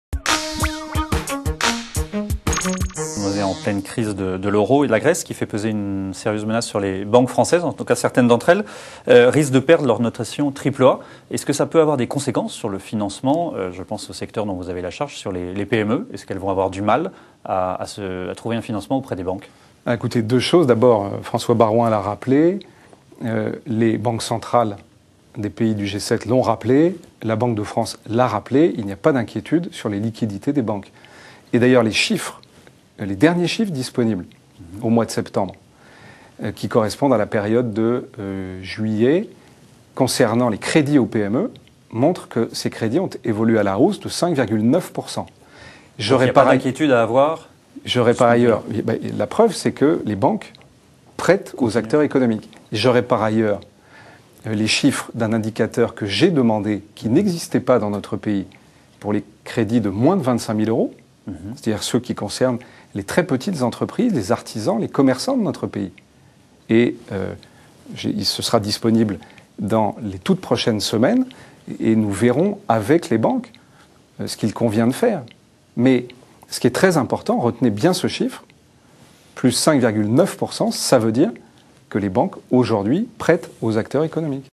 Frédéric Lefebvre
Affaire Bourgi: "Chacun sait que ce ne sont pas des pratiques du président de la République"